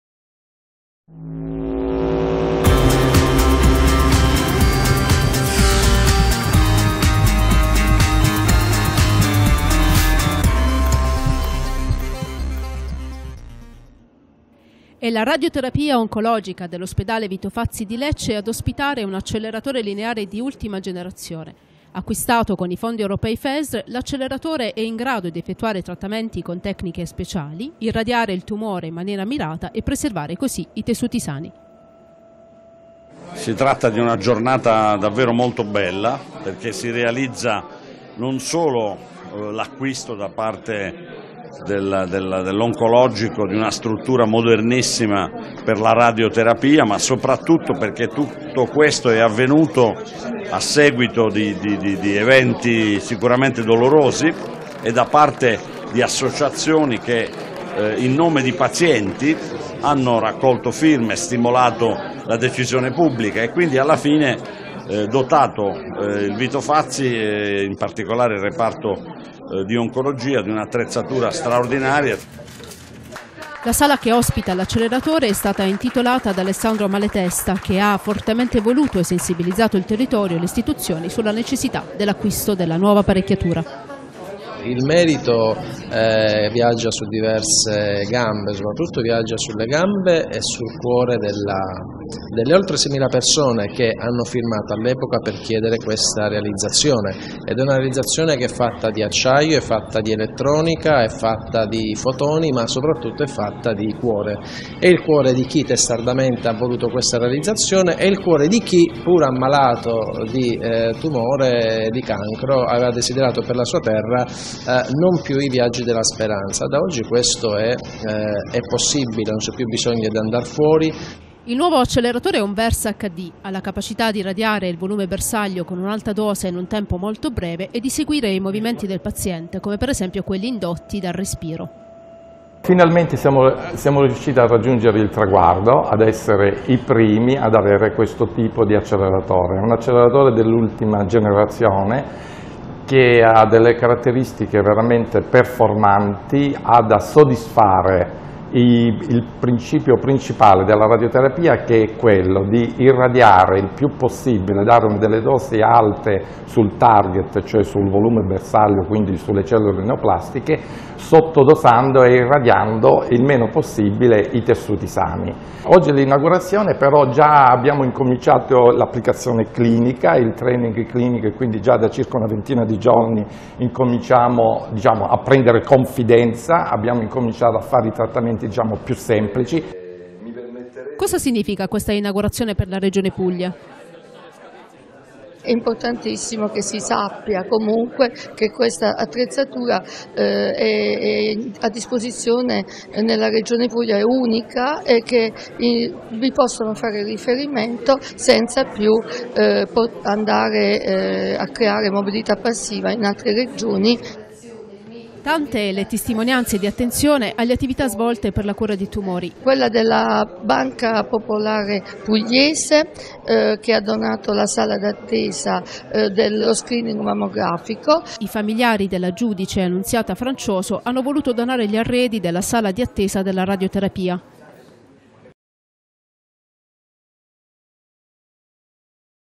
Acceleratore lineare_Lecce
E' la Radioterapia Oncologica dell'ospedale Vito Fazzi di Lecce, diretta dal dottor Mario Santantonio, ad ospitare un nuovo acceleratore lineare Versa HD di ultima generazione.
Servizio a cura di Roberto Sibilano, Micaela Abbinante e Sonia Giausa.